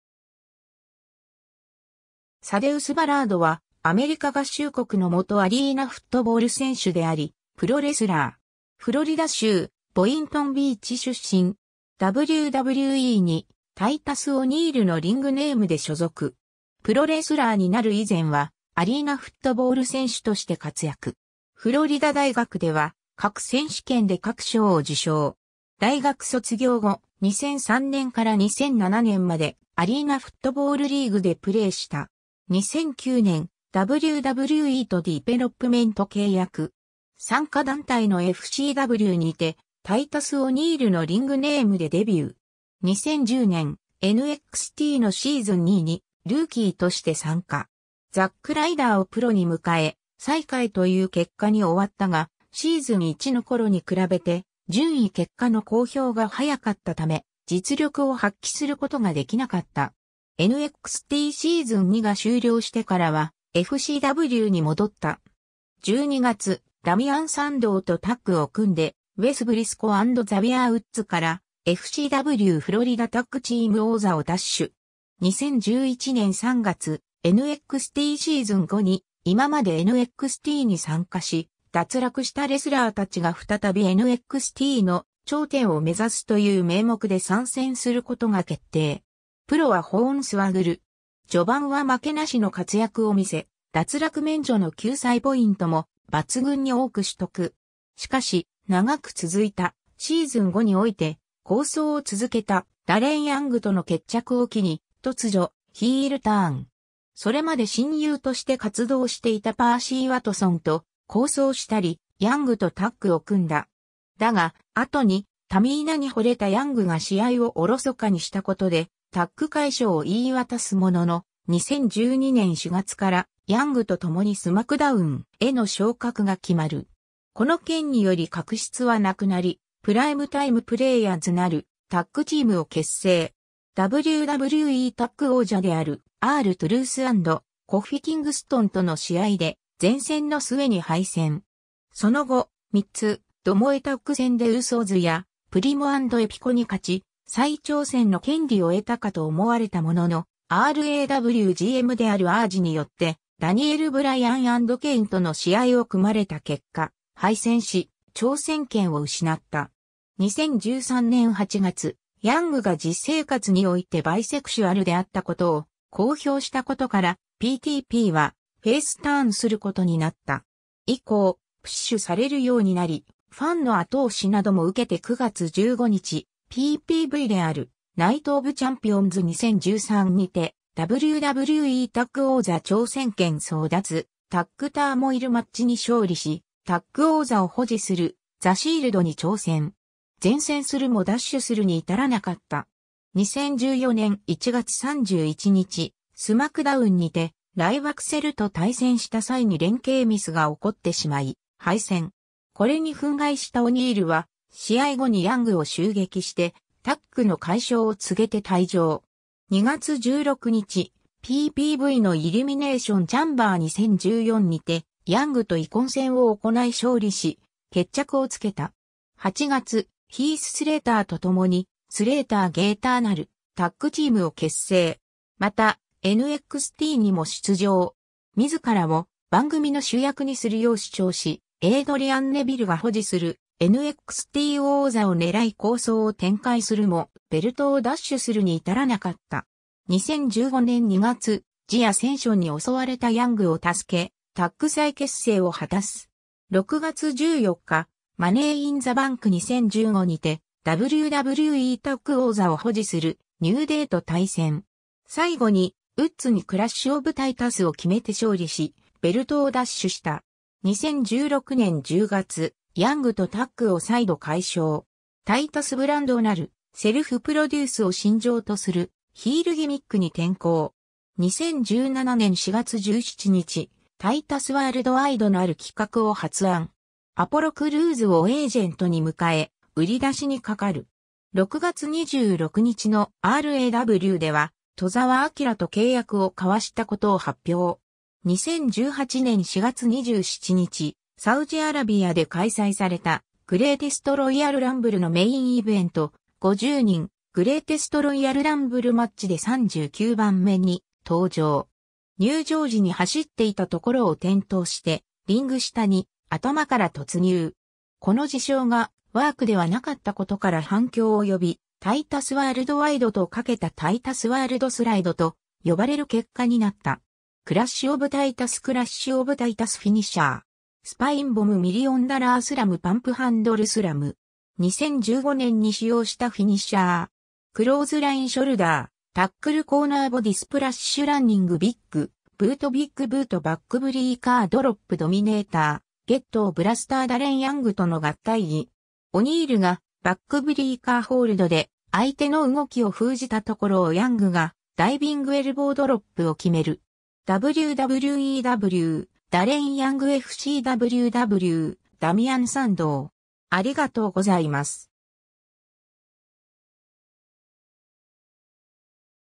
サデウス・バラード
サデウス・バラード（Thaddeus Michael Bullard Sr.、1977年4月29日 - ）は、アメリカ合衆国の元アリーナフットボール選手であり、プロレスラー。
フロリダ州ボイントンビーチ出身。
WWEにタイタス・オニール（Titus O'Neil）のリングネームで所属。
 プロレスラーになる以前は、アリーナフットボール選手として活躍。
フロリダ大学では各選手権で各賞を受賞。
大学卒業後、2003年から2007年までアリーナフットボールリーグでプレーした。
 2009年、WWEとディペロップメント契約。
傘下団体のFCWにてタイタス・オニール（Titus O'Neil）のリングネームでデビュー。
 2010年、NXTのシーズン2にルーキーとして参加。
ザック・ライダーをプロに迎え、最下位という結果に終わったが、シーズン1の頃に比べて順位結果の公表が早かったため、実力を発揮することができなかった。
NXTシーズン2が終了してからはFCWに戻った。
12月、ダミアン・サンドウとタッグを組んでウェス・ブリスコ & ザビアー・ウッズからFCWフロリダタッグチーム王座を奪取。
 2011年3月、NXTシーズン5に今までNXTに参加し、脱落したレスラーたちが再びNXTの頂点を目指すという名目で参戦することが決定。
プロはホーンスワグル。
序盤は負けなしの活躍を見せ、脱落免除の救済ポイントも抜群に多く取得。
しかし、長く続いたシーズン5において、抗争をつづけたダレン・ヤングとの決着（勝利）を機に突如ヒールターン。
それまで親友として活動していたパーシー・ワトソンと抗争したり、ヤングとタッグを組んだ。
だが、後にタミーナに惚れたヤングが試合をおろそかにしたことでタッグ解消を言い渡すものの、2012年4月からヤングとともにSmackDown!への昇格が決まる。
この件により確執は無くなり、プライムタイム・プレイヤーズ（Primetime Players）なるタッグチームを結成。
WWEタッグ王者であるR・トゥルース & コフィ・キングストンとの試合で善戦の末に敗戦。
その後、3つ巴タッグ戦でウーソズやプリモ & エピコに勝ち、再挑戦の権利を得たかと思われたものの、RAW GMであるAJによってダニエル・ブライアン & ケインとの試合を組まれた結果敗戦し、挑戦権を失った。
 2013年8月、ヤングが実生活においてバイセクシュアルであったことを公表した事からPTPはフェイスターンすることになった。
以降、プッシュされるようになり、ファンの後押しなども受けて9月15日、PPVであるNight of Champions 2013にてWWEタッグ王座挑戦権争奪タッグターモイルマッチに勝利し、タッグ王座を保持するザ・シールドに挑戦。
善戦するも奪取するに至らなかった。
 2014年1月31日、SmackDownにてライバクセル（ライバック & カーティス・アクセル）と対戦した際に連携ミスが起こってしまい敗戦。
これに憤慨したオニールは試合後にヤングを襲撃してタッグの解消を告げて退場。
2月16日、PPVのElimination Chamber 2014にてヤングと遺恨戦を行い勝利し、決着をつけた。
8月、ヒース・スレイターと共にスレイター・ゲーター（Slater-Gator）なるタッグチームを結成。
また、NXTにも出場。
自らを番組の主役にするよう主張し、エイドリアン・ネヴィルが保持するNXT王座を狙い抗争を展開するもベルトを奪取するに至らなかった。
 2015年2月、ジ・アセンションに襲われたヤングを助け、タッグ再結成を果たす。
6月14日、Money in the Bank 2015にてWWEタッグ王座を保持するニュー・デイ（ビッグ・E & エグザビアー・ウッズ）と対戦。
最後にウッズにクラッシュ・オブ・タイタスを決めて勝利し、ベルトを奪取した。
 2016年10月、ヤングとタッグを再度解消。
タイタス・ブランド（The Titus Brand）なるセルフプロデュースを信条とするヒールギミックに転向。
 2017年4月17日、タイタス・ワールドワイド（Titus Worldwide）なる企画を発案。
アポロ・クルーズをエージェントに迎え、売出しにかかる。
6月26日のRAWでは戸澤陽と契約を交わしたことを発...